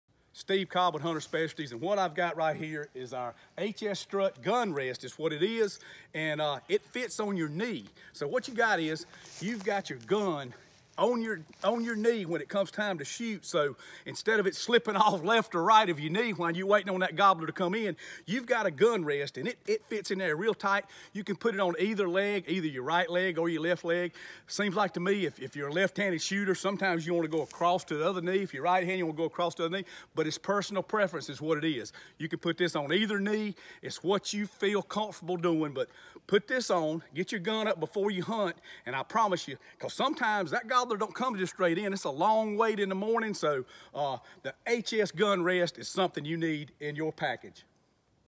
How to Use the HS Strut Knee Pad Gun Rest
Being at the ready at all times during a turkey setup can be a challenge. Here's a little tip from HS Field Staffer Steve Cobb on how to stay comfortable but ready for that ol' gobbler to come into range.